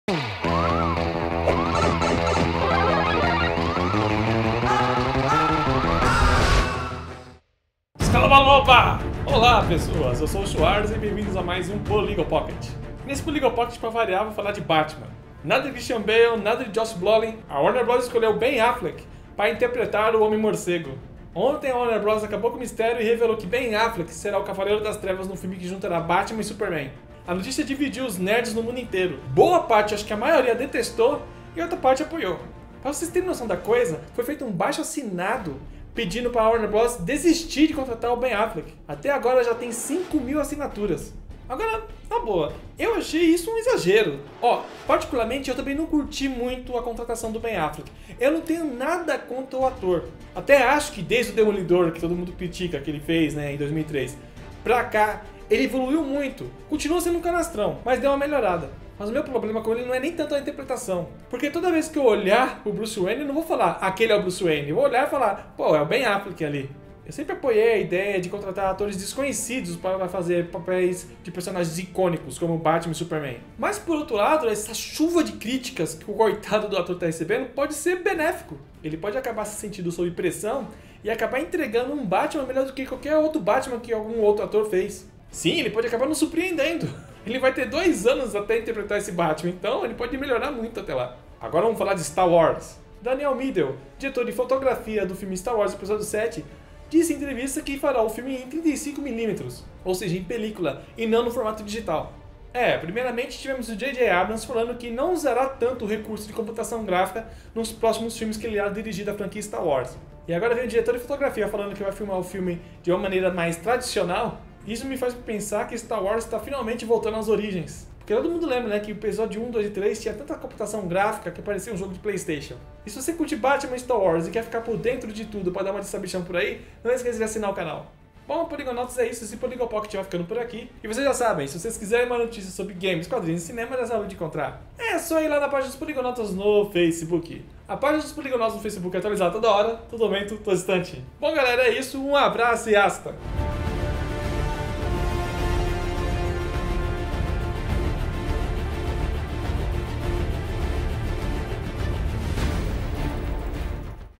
Ben Affleck é o Novo Batman, Star Wars | PoligoPocket
Faaaaaala meus fofos! Bem-vindo a mais um PoligoPocket!

E nesse programa o Schwarza comenta a contratação do ator Ben Aflfleck para o papel de Batman no cinema e Star Wars.